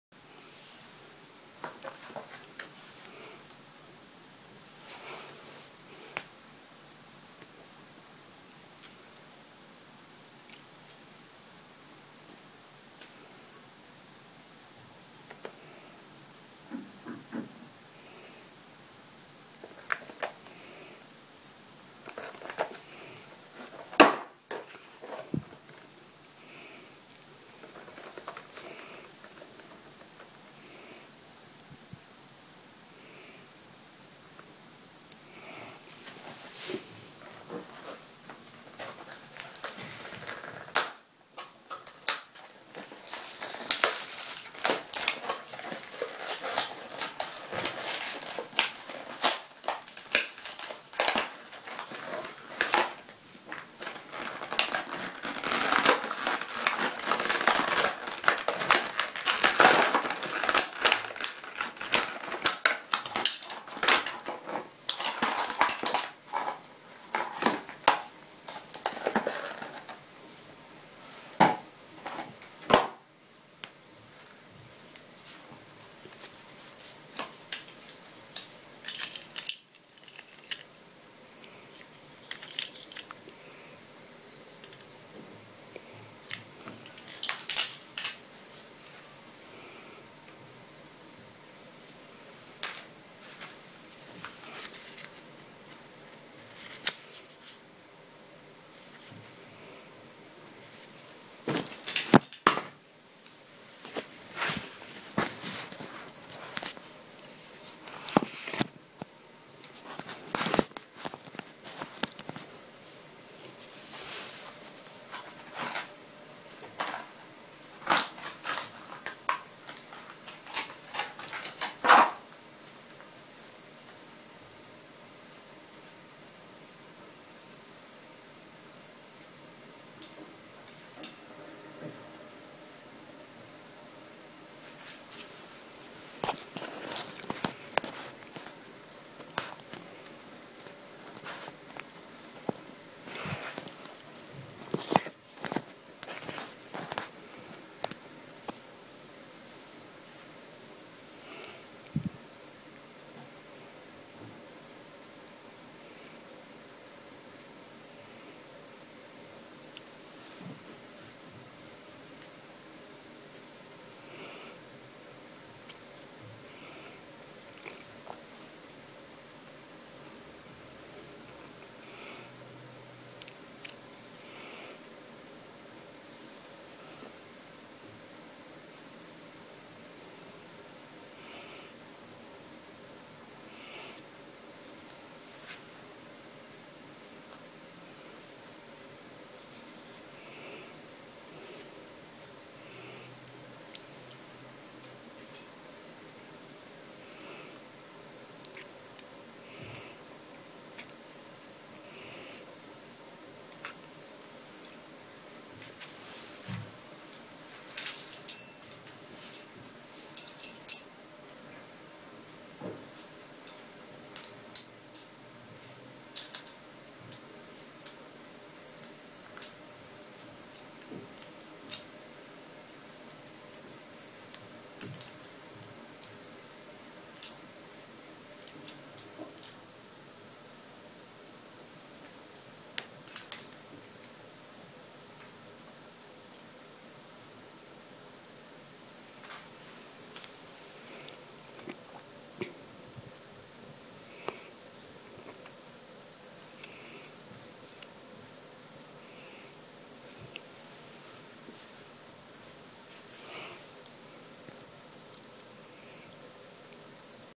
big bang pegasus F:D UNBOXING
hey guys today i unboxing the wing horse hope you will enjoy this unboxing if you did plz like comment subscribe & request some battles in the comments below with BBP & sorry for the camera